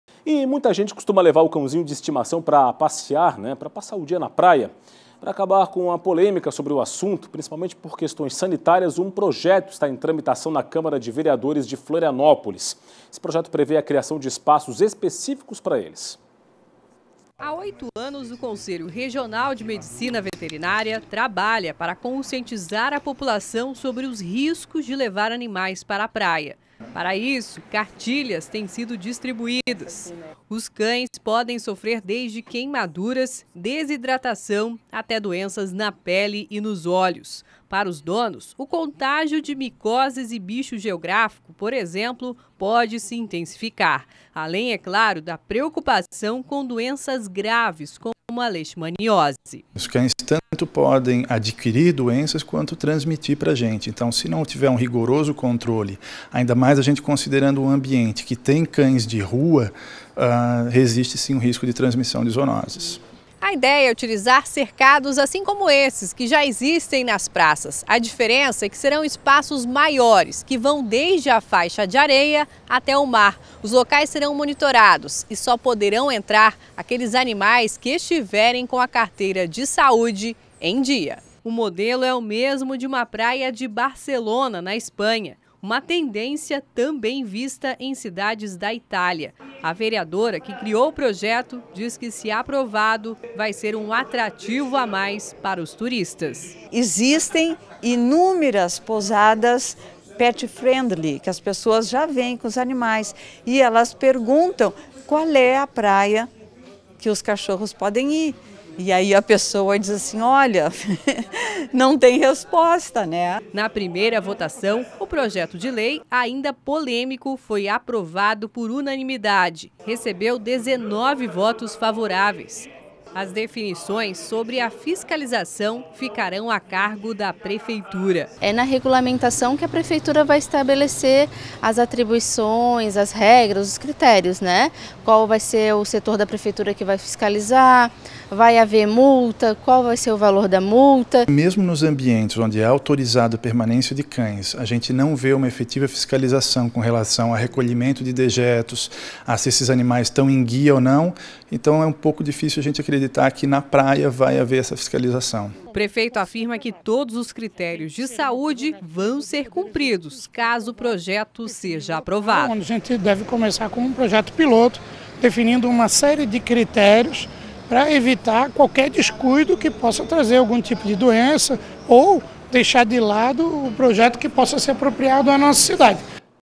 Projeto de lei quer propor espaços específicos aos animais de estimação nas praias
Muita gente costuma levar o cãozinho de estimação para passar o dia na praia. Para acabar com a polêmica sobre o assunto, principalmente por questões sanitárias, um projeto está em tramitação na Câmara de Vereadores de Florianópolis e prevê a criação de espaços específicos para eles.